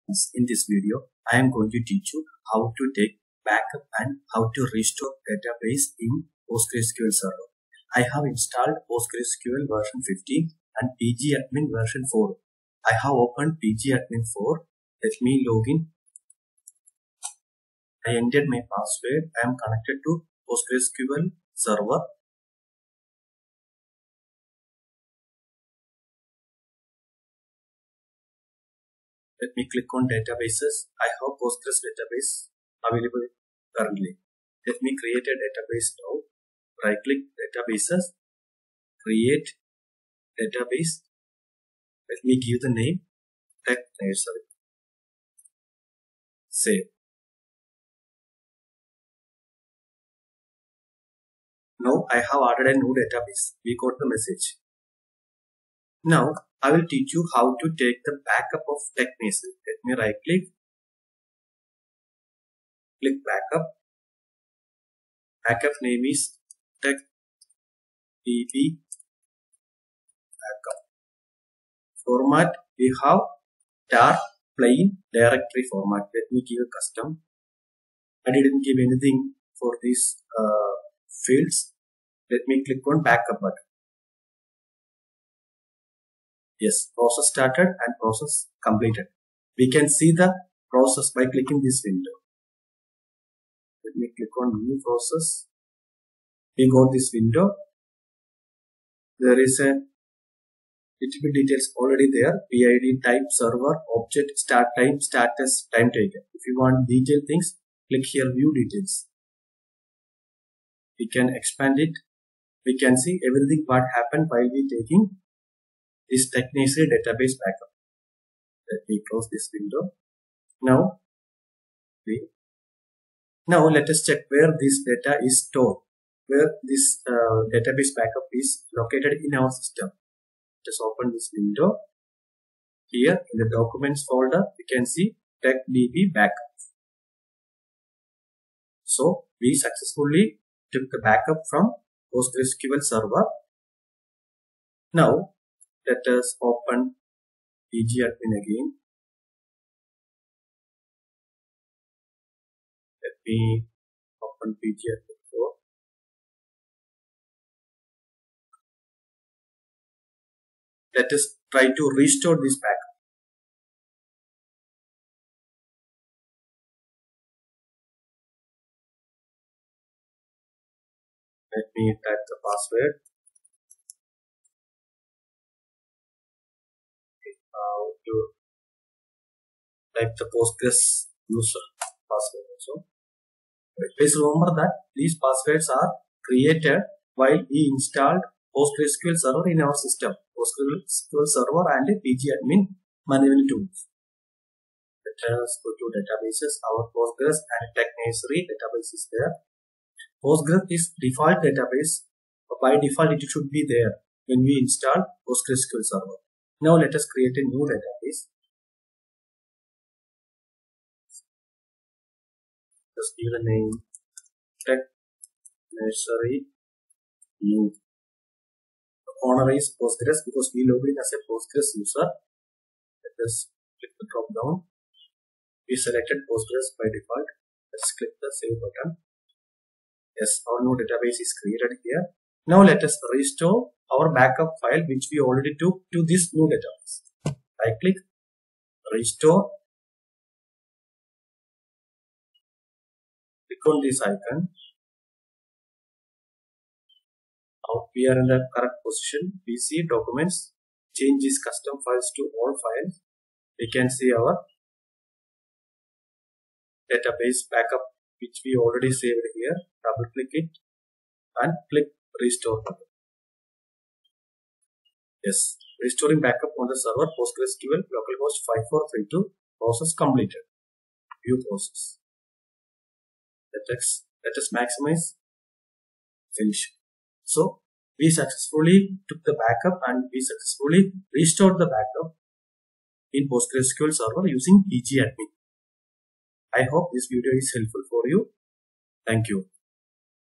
backup restore postgreSQL 15 database in pgadmin 4 postgreSQL tutorial explained
How to open pgAdmin 4
what is postgres user password?
How we can create postgres user password?
How to create database in postgreSQL 15 Server
How to restore postgreSQL database
How to backup postgreSQL database